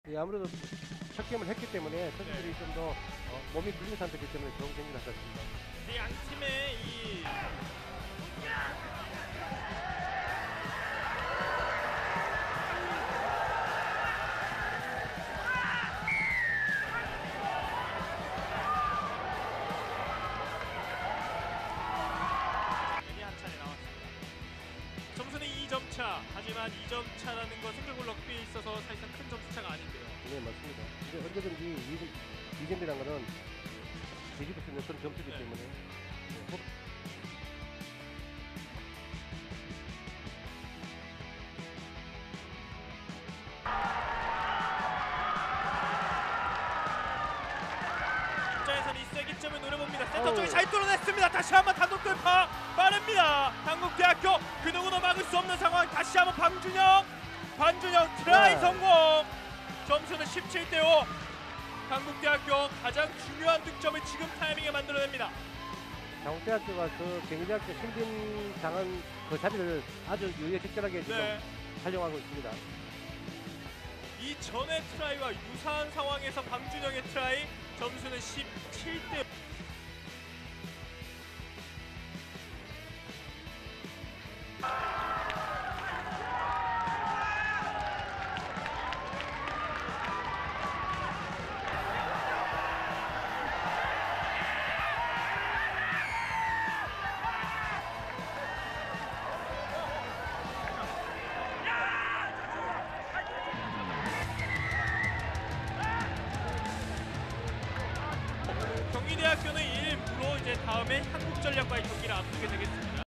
[2016년도 전국 7인제 럭비대회] 단국대vs경희대 (2016.07.8) 하이라이트
[2016년도 전국 7인제 럭비대회] 단국대vs경희대 (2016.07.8) 하이라이트 인터넷 방송입니다.